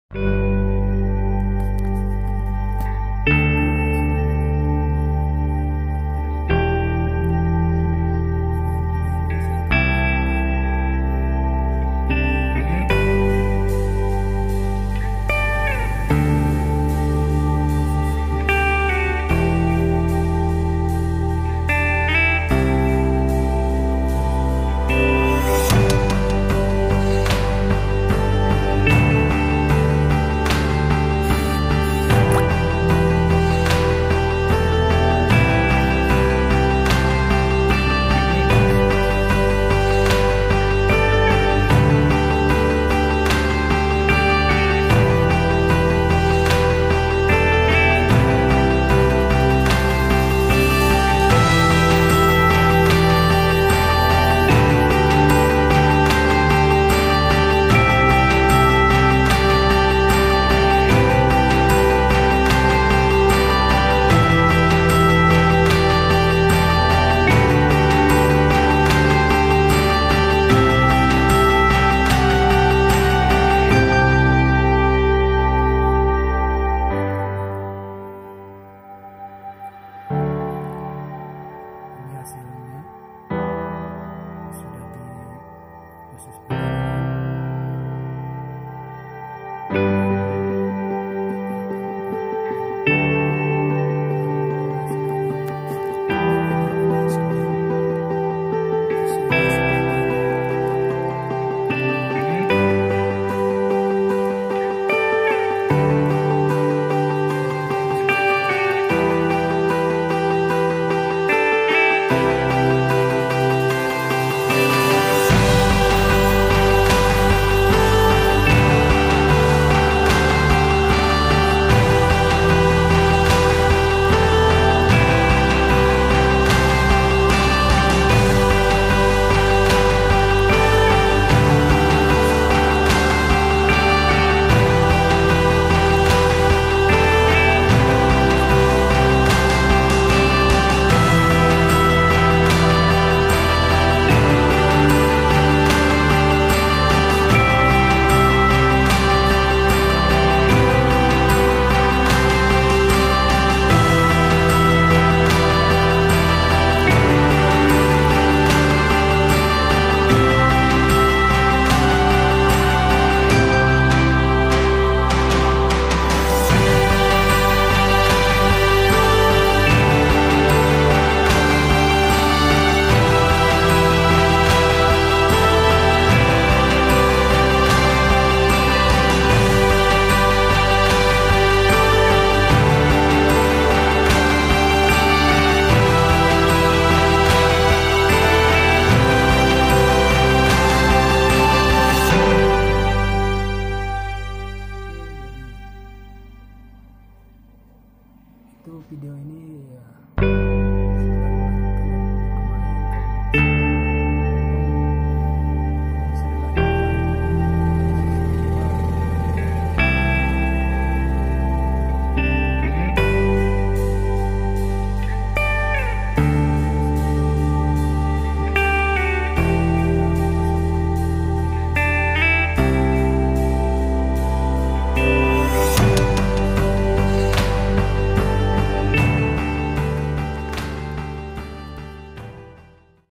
Lanjutan setelah biji kopi melalui proses penggilingan
Siap untuk di pakin Hasil sesudah penggilangan